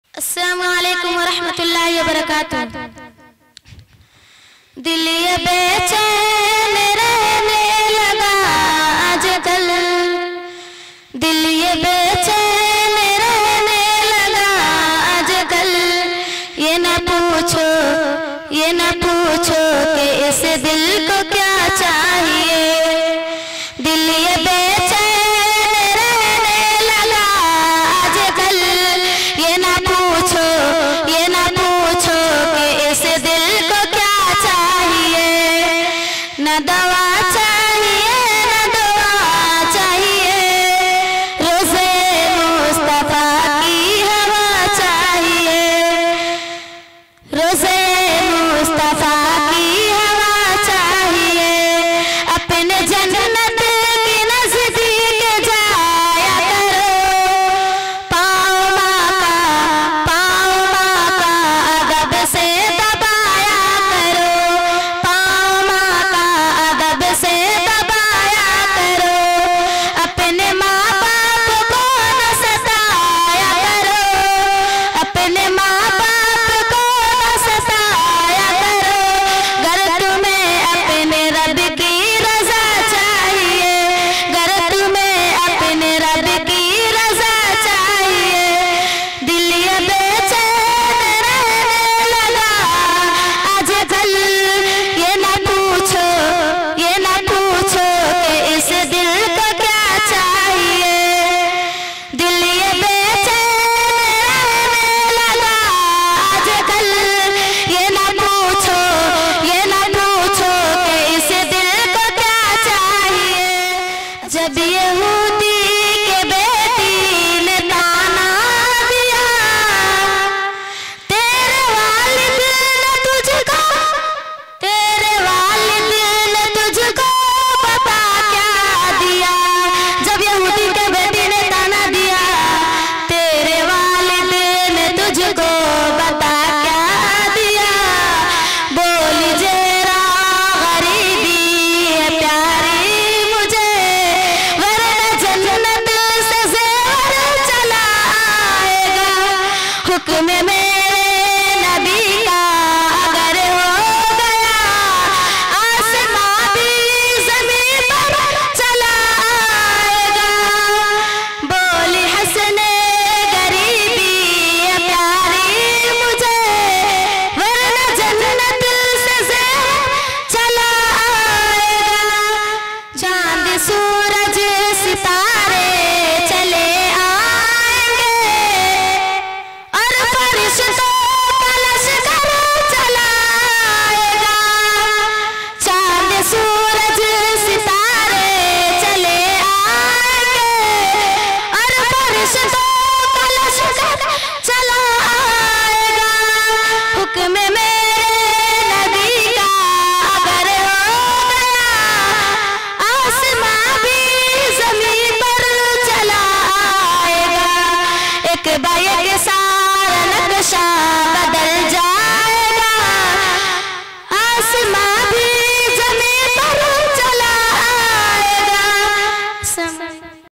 बिलकुल कारी सुहैल की तरह / Israt Khatoon / Jamia Darul Muminat Sathi | 2023
Nazim E Madrasa :- Qari Habibur Rahman Jamai

Sadar E Jalsa :-  Maulana Allauddin Qasmi

Venue:- Jamia Darul Muminat Sathi

Vedio Recording And Uploading By:-s H Mushaira Media
 Mr. Serazul Haque (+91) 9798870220